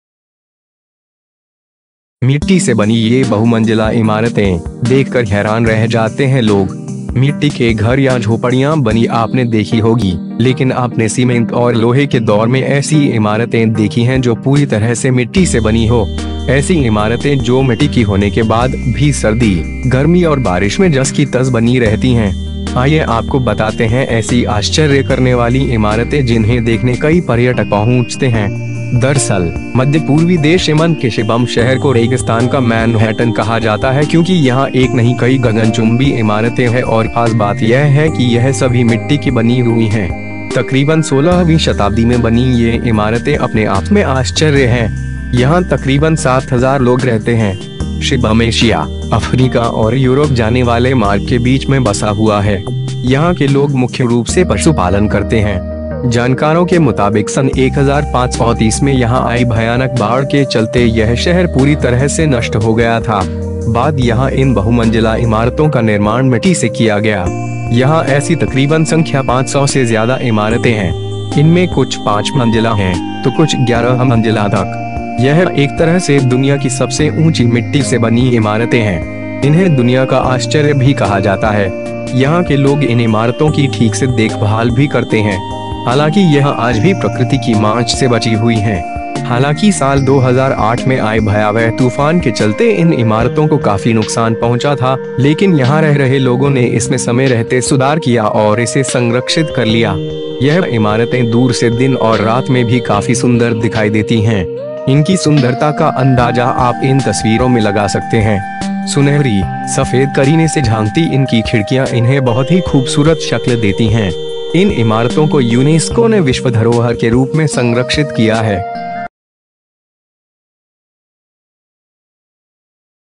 मिट्टी से बनी ये बहुमंजिला इमारतें, देखकर हैरान रह जाते हैं लोग
for mouth watering Indian Food Recipes मिट्टी से बनी ये बहुमंजिला इमारतें, देखकर हैरान रह जाते हैं लोग

मिट्टी के घर या झोपड़ियां बनी आपने देखी होगी, लेकिन आपने सीमेंट और लोहे के दौर में ऐसी इमारतें देखी हैं जो पूरी तरह से मिट्टी से बनी हो. ऐसी इमारतें जो मिट्टी की होने के बाद भी सर्दी, गर्मी और बारिश में जस की तस बनीं रहती हैं. आइए आपको बताते हैं ऐसी आश्‍चर्य करने वाली इमारतें जिन्‍हें देखने कई पर्यटक पहुंचते हैं.

दरअसल, मध्‍य पूर्वी देश यमन के शिबम शहर को रेगिस्‍तान का मैनहैट्टन कहा जाता है क्‍योंकि यहां एक नहीं कई गगन चुम्‍बी इमारतें है और खास बात यह है कि यह सभी मिट्टी की बनीं हुई हैं.

तकरीबन 16वीं शताब्‍दी में बनीं ये इमारतें अपने आप में आश्‍चर्य हैं. यहां तकरीबन 7000 लोग रहते हैं.

शिबम एशिया, अफ्रीका और यूरोप जाने वाले मार्ग के बीच में बसा हुआ है. यहां के लोग मुख्‍य रूप से पशुपालन करते हैं.

जानकारों के मुताबिक सन् 1530 में यहां आई भयानक बाढ़ के चलते यह शहर पूरी तरह से नष्‍ट हो गया था. बाद यहां इन बहुमंजिला इमारतों का निर्माण मिट्‌टी से किया गया.

यहां ऐसी तकरीबन संख्या 500 से ज्यादा इमारतें हैं. इनमें कुछ पांच मंजिला हैं, तो कुछ 11 मंजिला तक.

यह एक तरह से दुनिया की सबसे ऊंची मिट्टी से बनीं इमारतें हैं. इन्‍हें दुनिया का आश्‍चर्य भी कहा जाता है. यहां के लोग इन इमारतों की ठीक से देखभाल भी करते हैं. हालांकि यह आज भी प्रकृति की मार से बची हुई हैं.

हालांकि साल 2008 में आए भयावह तूफान के चलते इन इमारतों को काफी नुकसान पहुंचा था, लेकिन यहां रह रहे लोगों ने इसमें समय रहते सुधार किया और इसे संरक्षित कर लिया.

यह इमारतें दूर से दिन और रात में भी काफी सुंदर दिखाई देती हैं. इनकी सुंदरता का अंदाजा आप इन तस्‍वीरों में लगा सकते हैं. सुनहरी, सफेद करीने से झांकती इनकी खिड़कियां इन्‍हें बहुत ही खूबसूरत शक्‍ल देती हैं. इन इमारतों को यूनेस्‍को ने विश्‍व धरोहर के रूप में संरक्षित किया है.